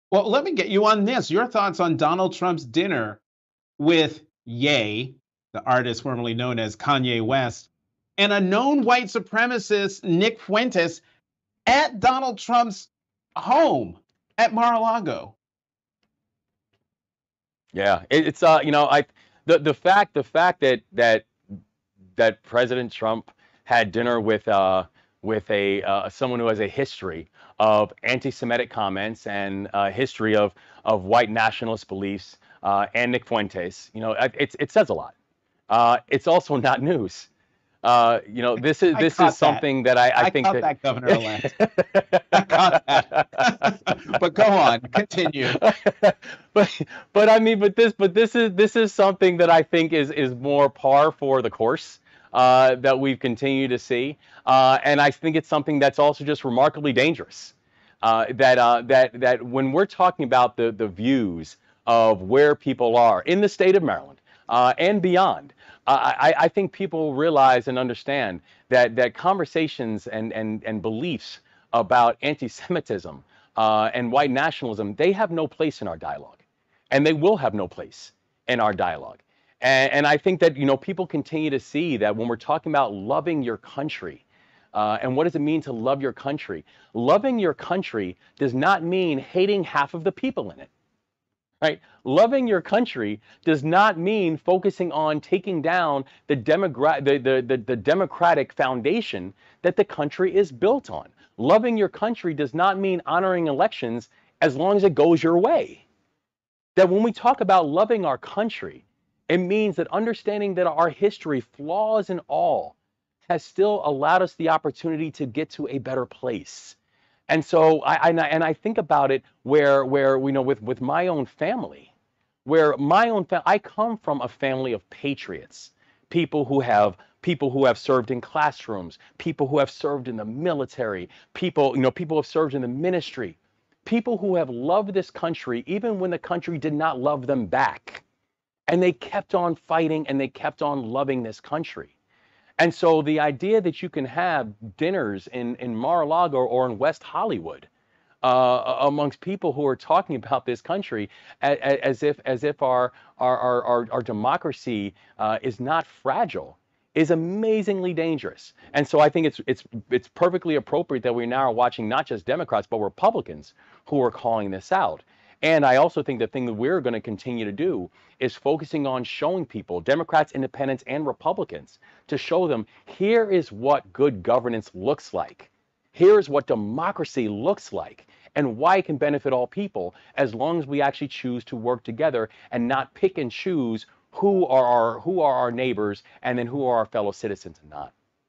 Moore calls Trump's dinner with Ye and Nick Fuentes "remarkably dangerous"
“The fact that President Trump had dinner with someone who has a history of anti-Semitic comments and a history of White Nationalist beliefs and Nick Fuentes, it says a lot. It's also not news... This is something that I think is more par for the course… I also think it’s something that’s remarkably dangerous. When we’re talking about the views of where people are, in the state of Maryland and beyond, I think people realize and understand that conversations and beliefs about anti-Semitism and White Nationalism, they have no place in our dialogue. And they will have no place in our dialogue.”- Wes Moore, Maryland Gov.-elect (D)